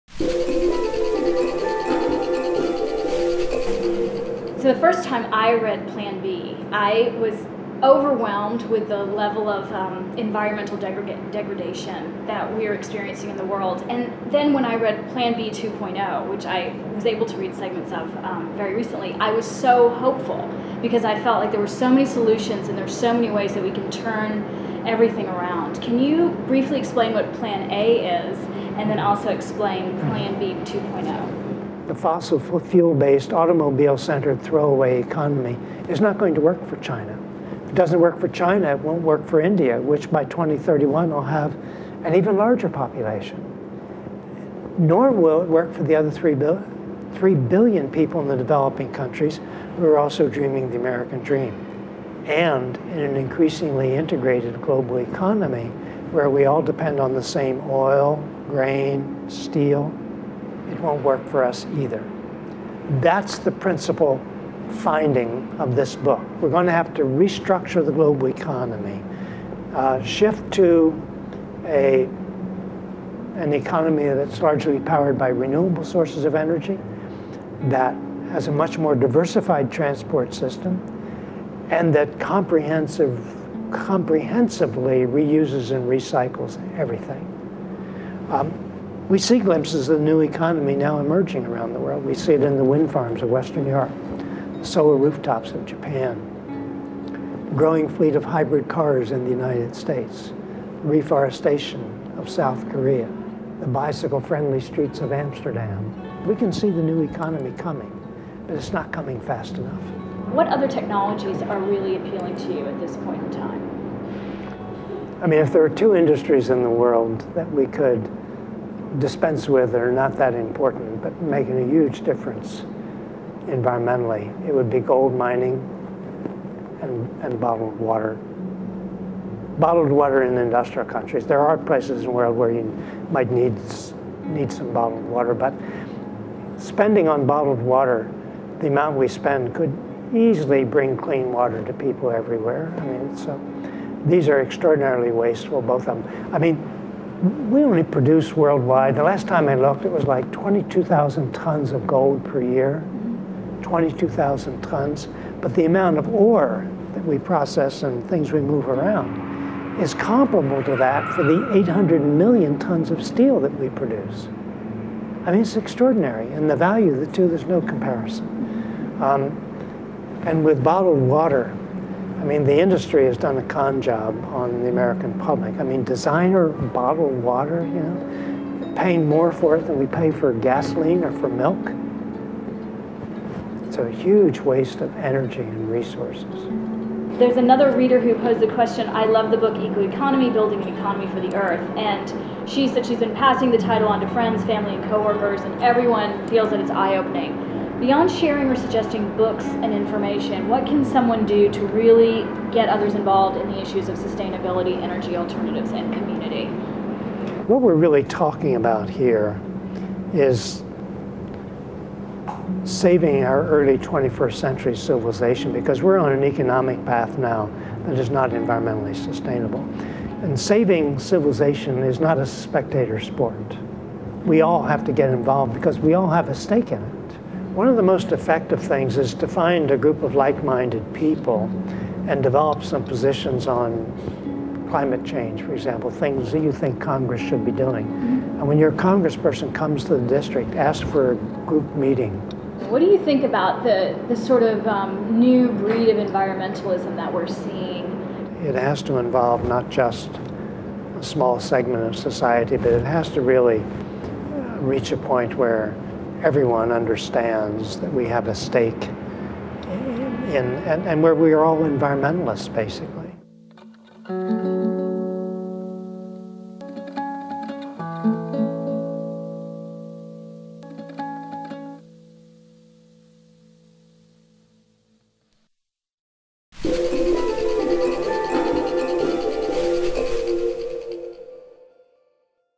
TreehuggerTV: A Conversation with Lester Brown
After a recent presentation at Demos in New York, our very own Simran Sethi sat down with Lester Brown to talk about his latest book "Plan B 2.0', what gold and bottled water have in common and what we can do to help the new economy rise. Brown is a renowned environmental thinker and president of the Earth Policy Institute which is a nonprofit research organization in Washington, D.C.